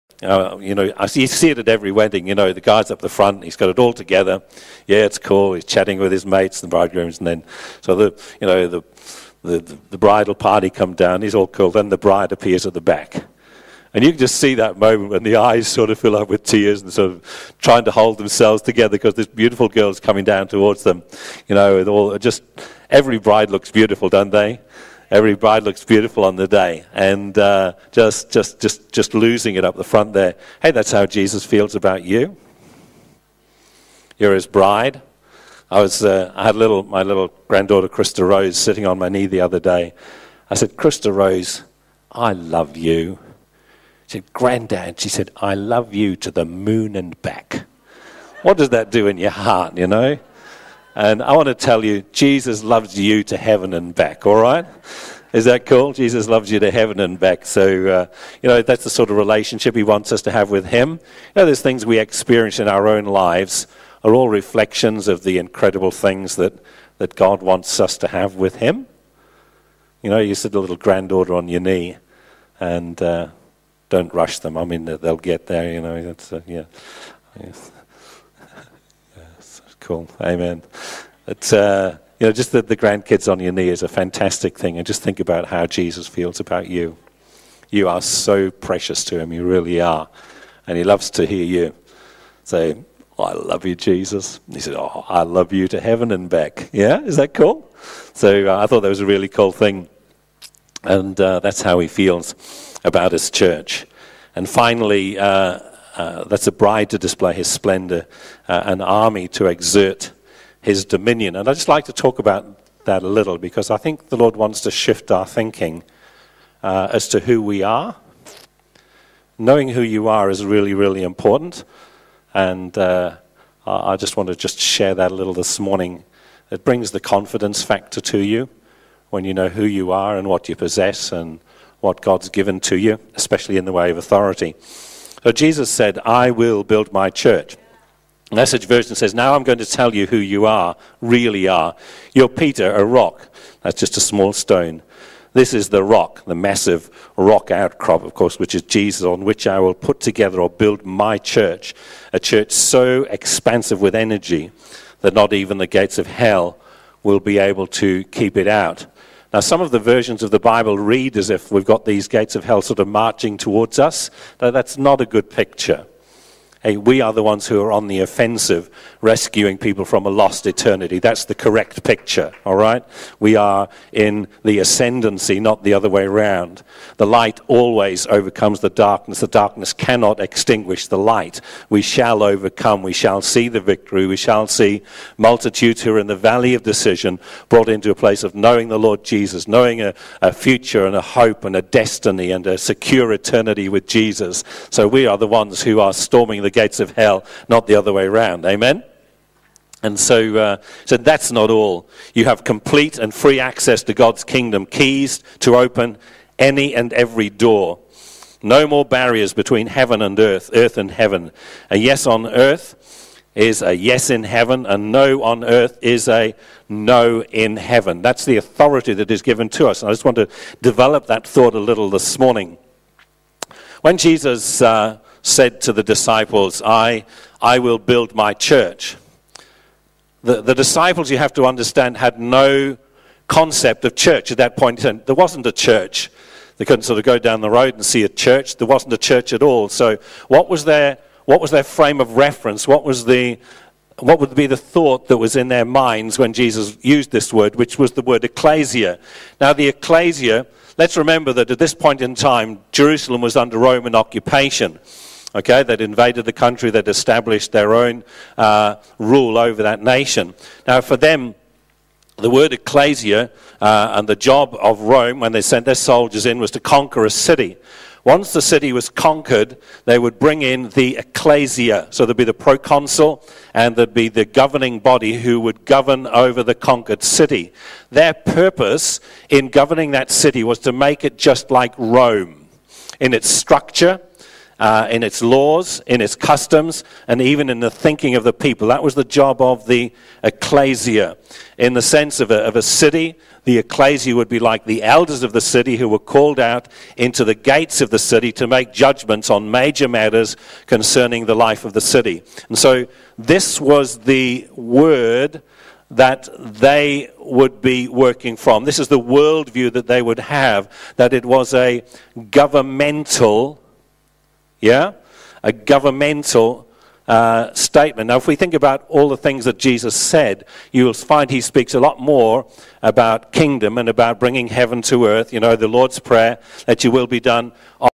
Ps Mike Knott: The Church Jesus Sees
Excerpts from a series of messages on how Jesus sees His Church